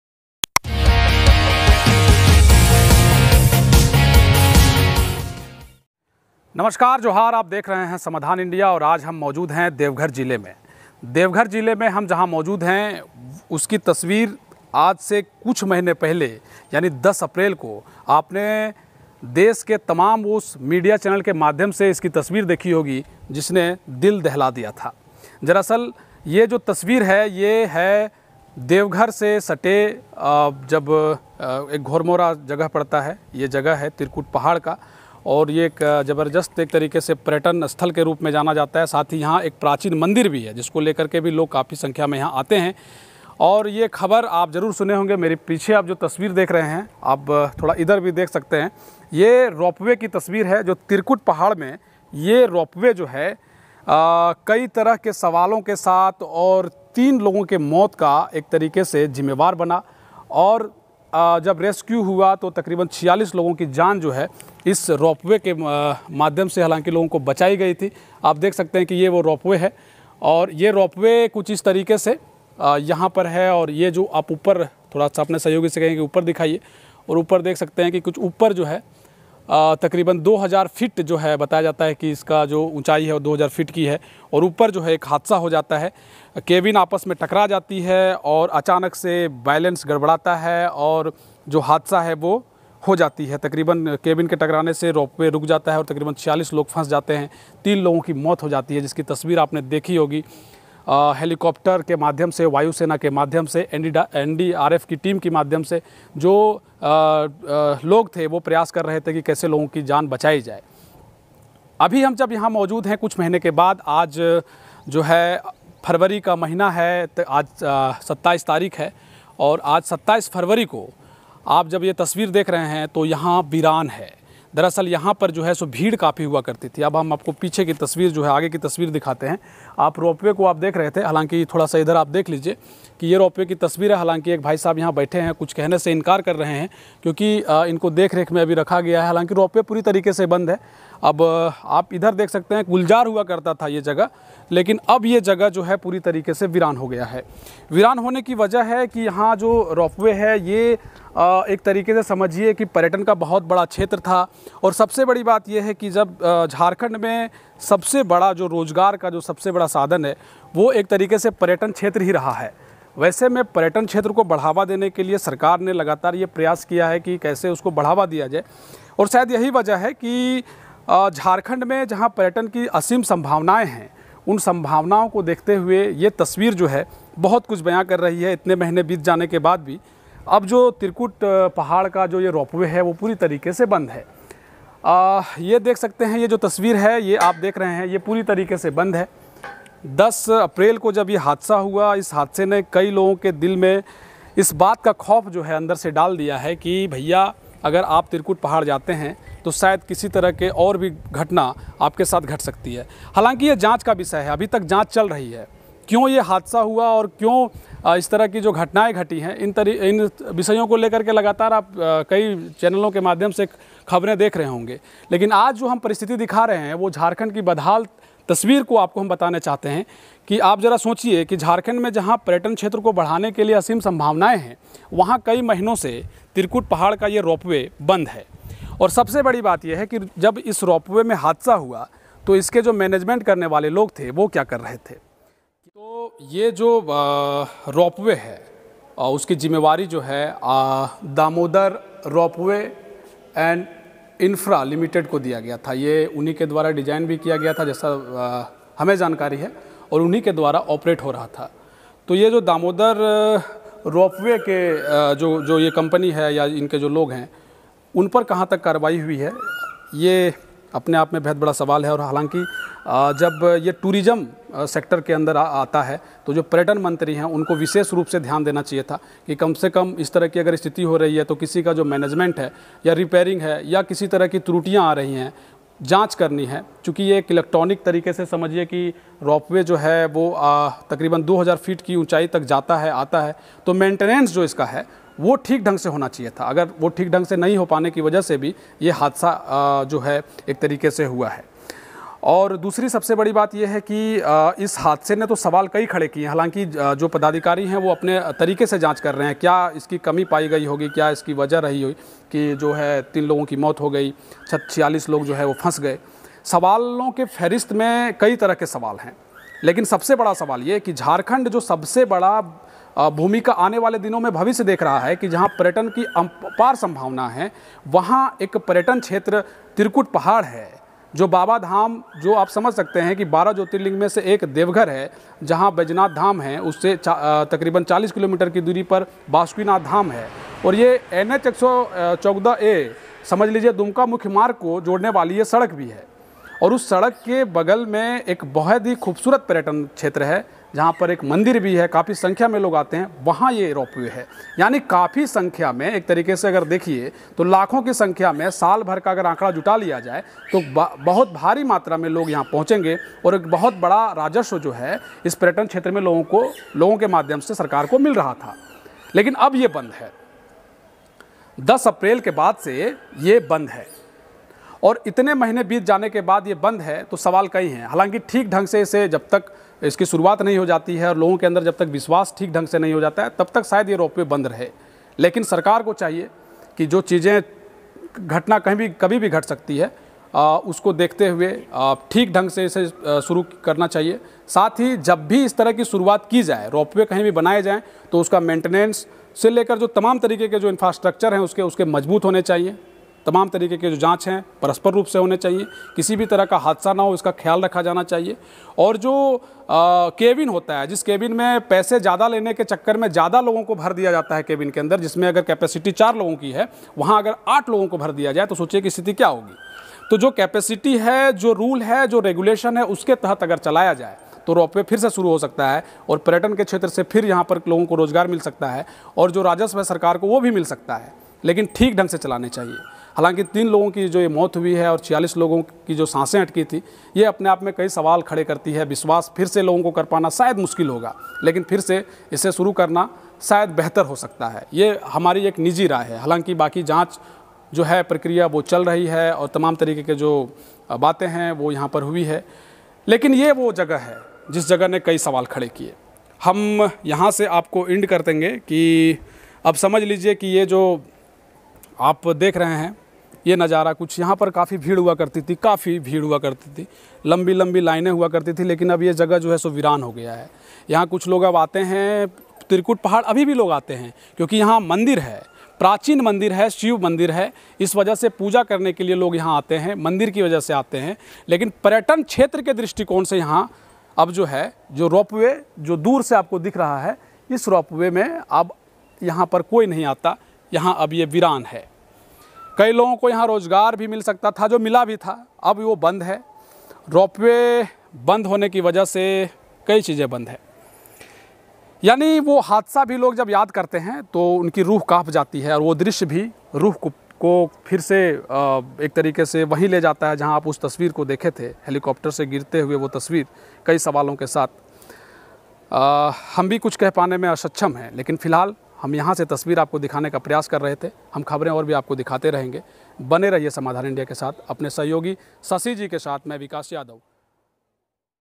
देवघर-त्रिकूट रोपवे हादसा कैसे हुआ था? किसकी थी गलती? क्या फिर से शुरू होगा रोपवे?जानिए
देवघर-त्रिकूट रोपवे हादसा के बाद वीरान है पर्वत,क्या फिर से शुरू होगा रोपवे?किसकी थी गलती?जानिए